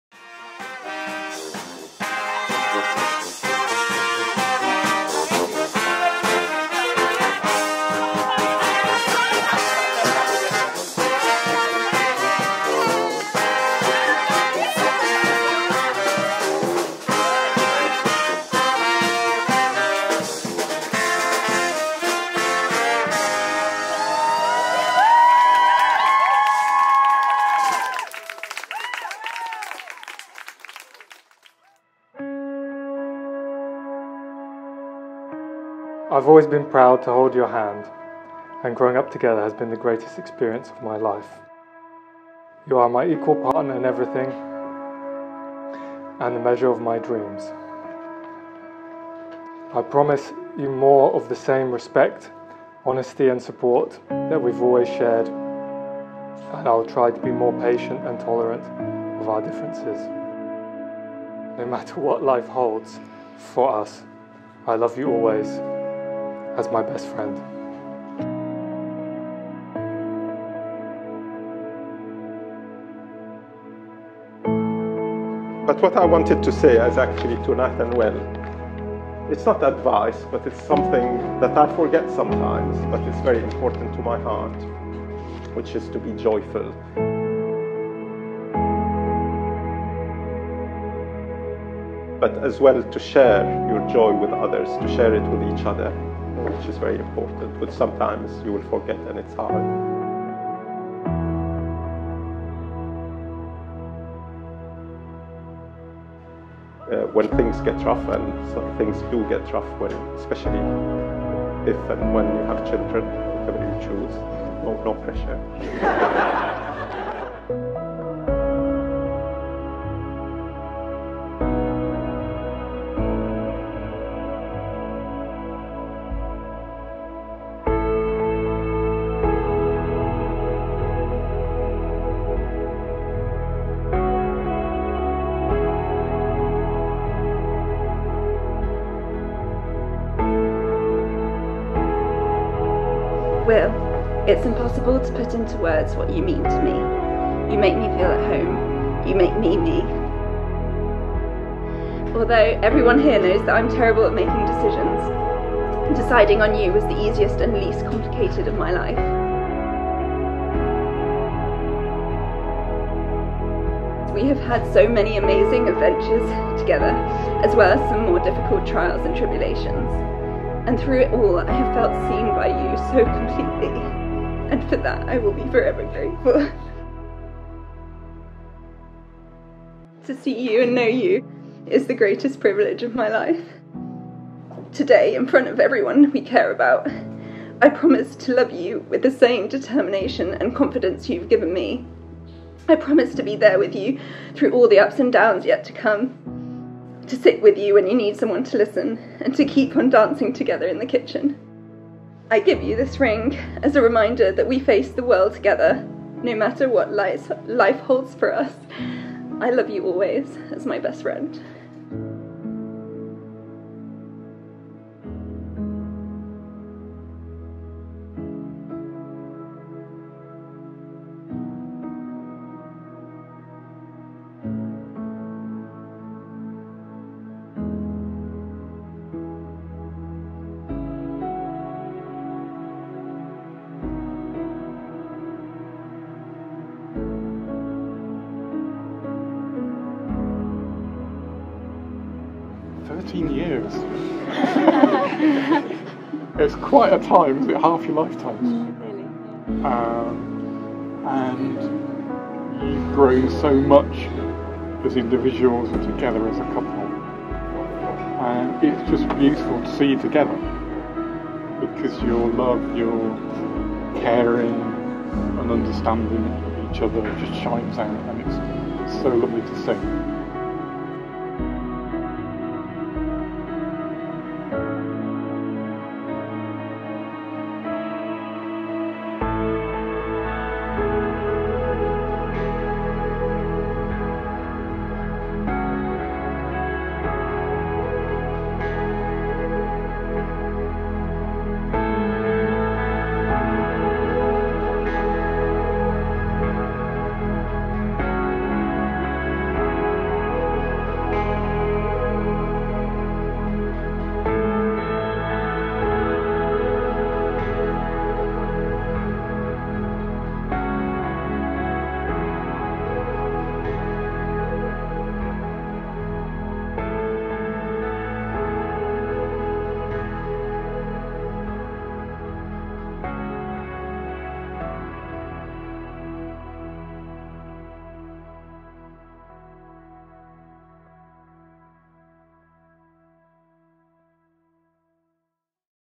Alma Tavern, Bristol / Brass Band Wedding Film
N&W's beautiful wedding ceremony at the Alma Tavern in Bristol, followed by a brass-band parade through the streets and a silent disco.